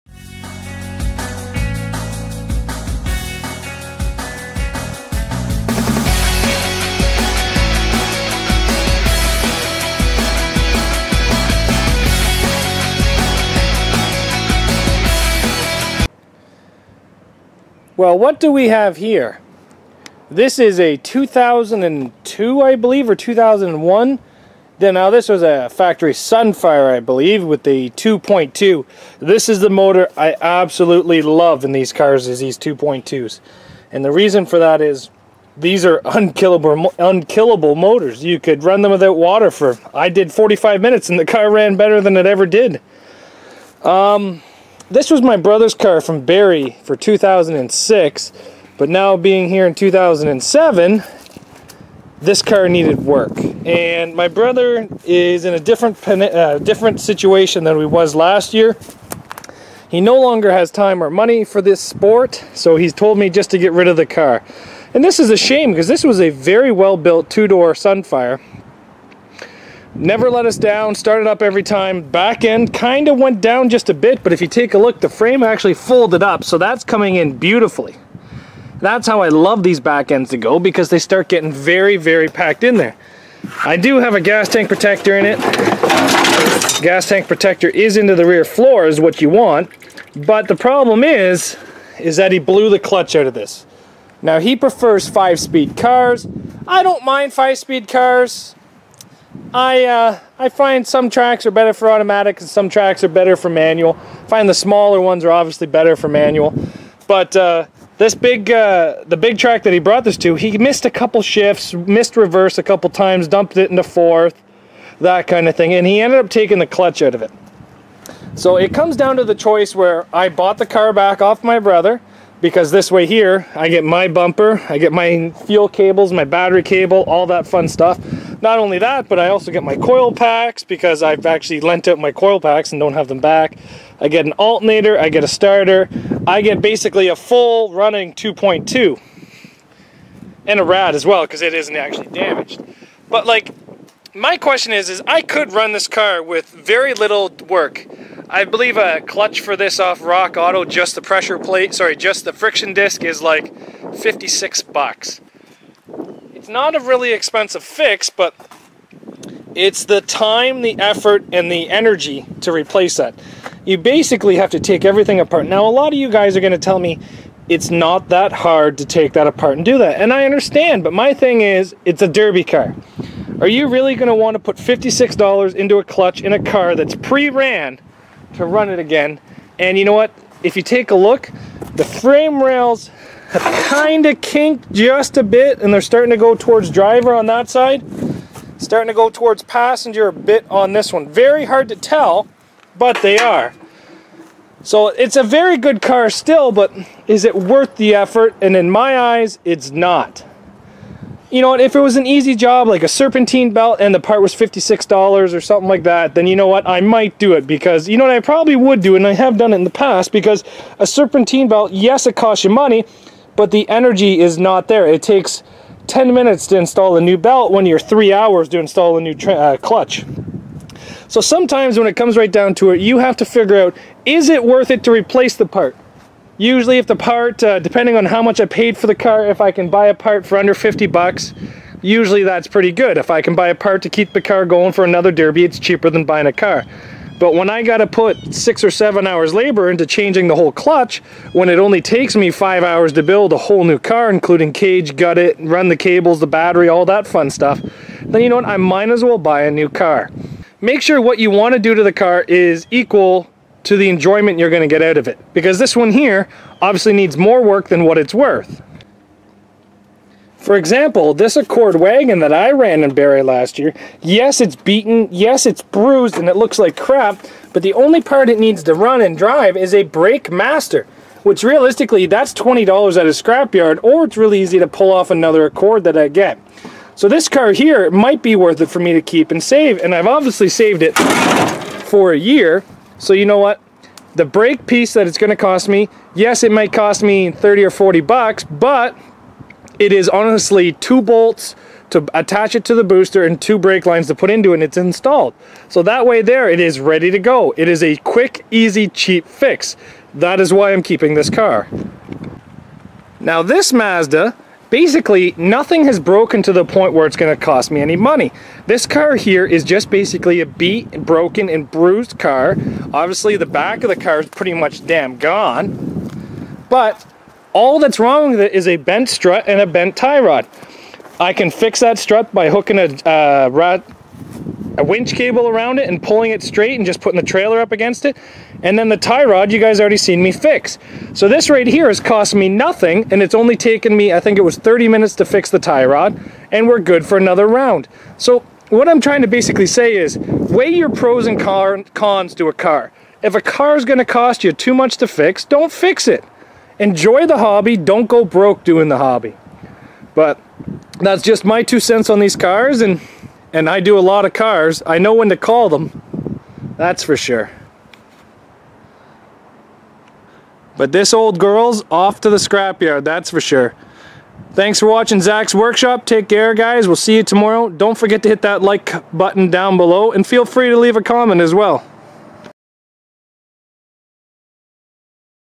save or not to save, when to fix or scrap a derby car
some times you have to call it , fix or scrap